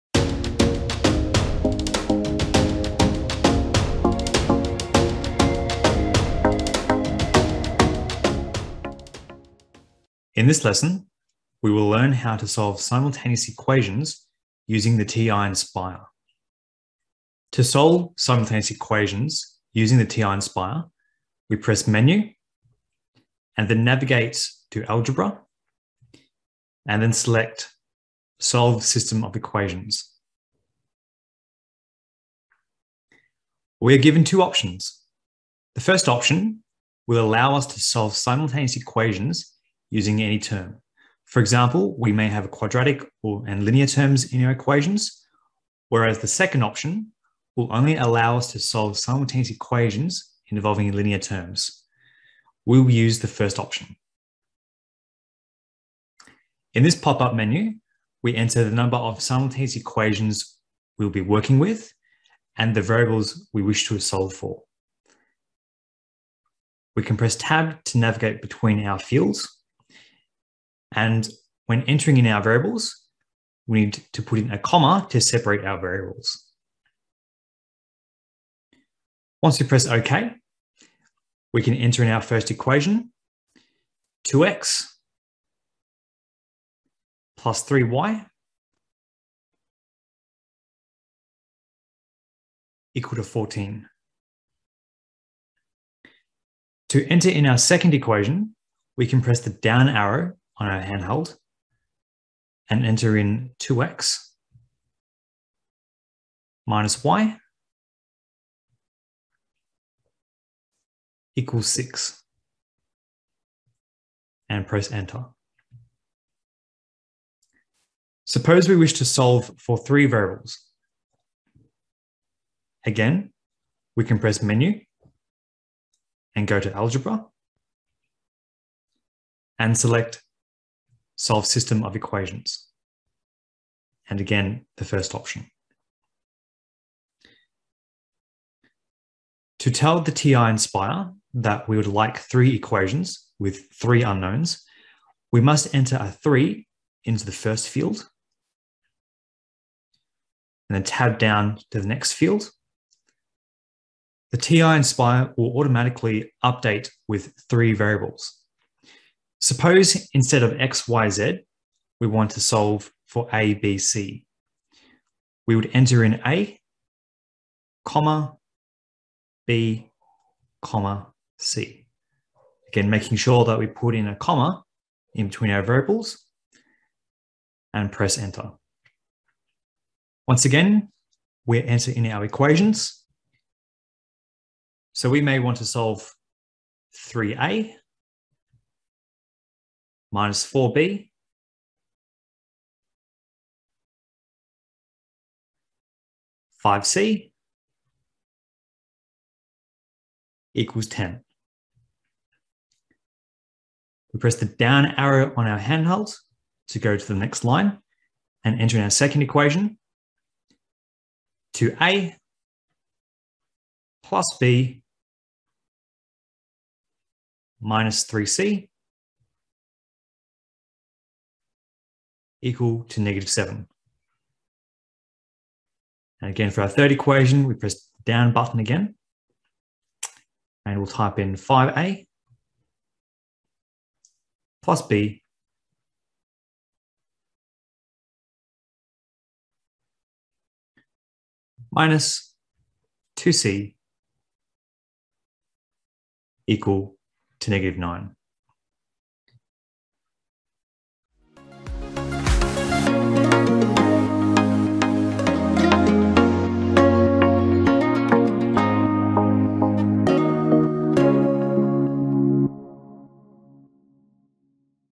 Solve Systems of Equations on the TI-Nspire CX II CAS Graphing Calculator
Explore how to solve simultaneous equations, or systems of equations, on the TI-Nspire CX II CAS graphing calculator, including how to solve for three variables.

Chapters
0:00 Intro
0:10 Solve Systems of Equations on the TI-Nspire CX II CAS Graphing Calculator
1:58 Solve for Three Variables on the TI-Nspire CX II CAS Graphing Calculator